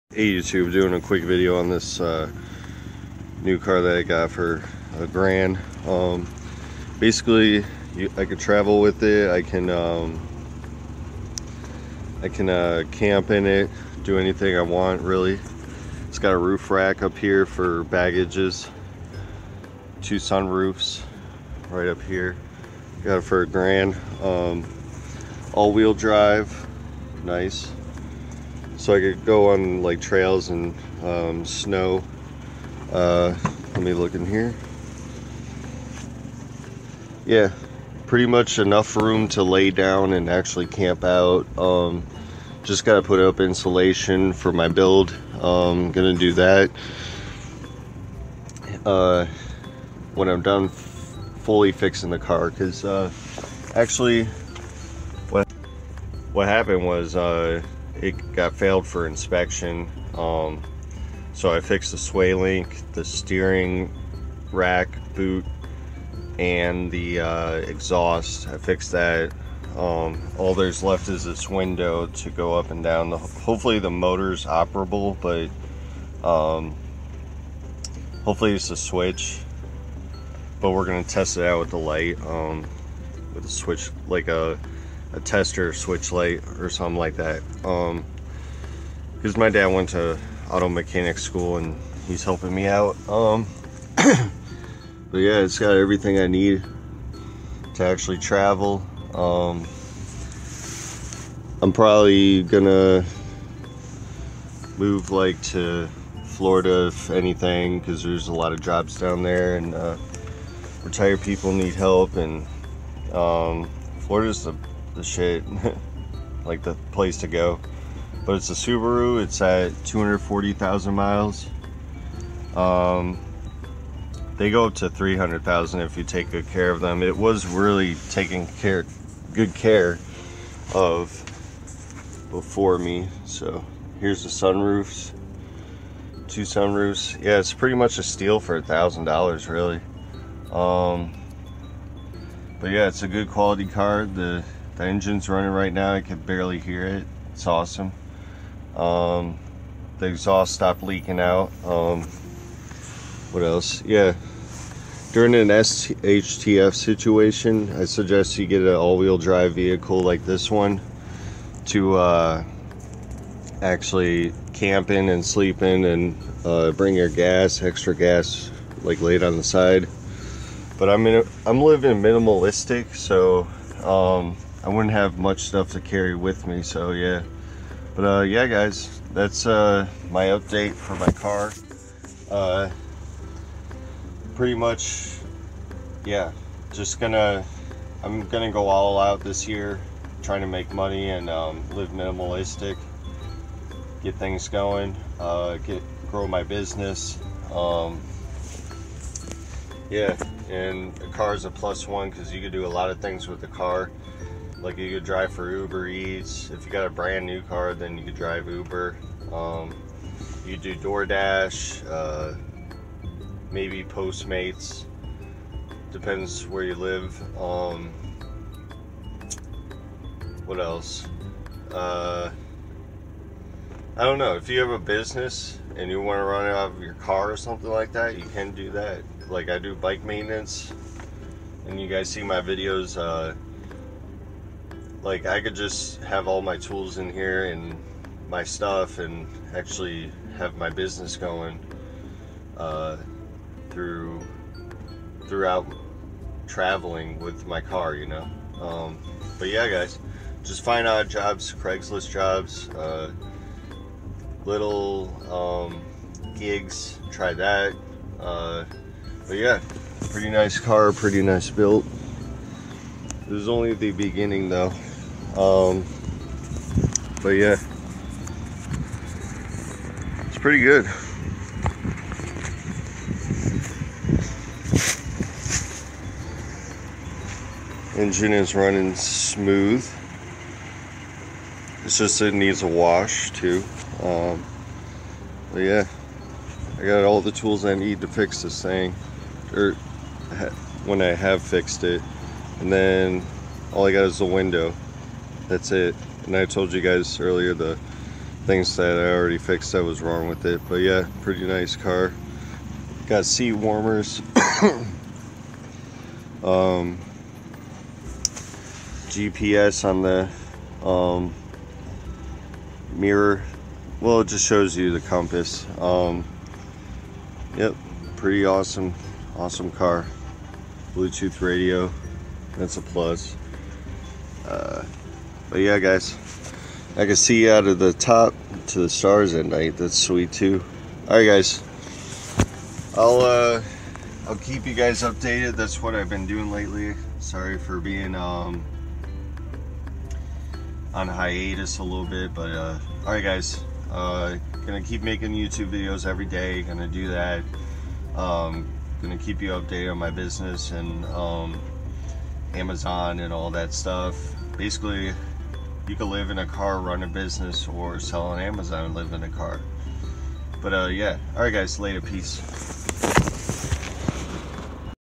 My New Car! For 1K (Update)
Been on hiatus for a little bit just showing what car i'll be using for camping, hiking, backpacking, mtb, building my business.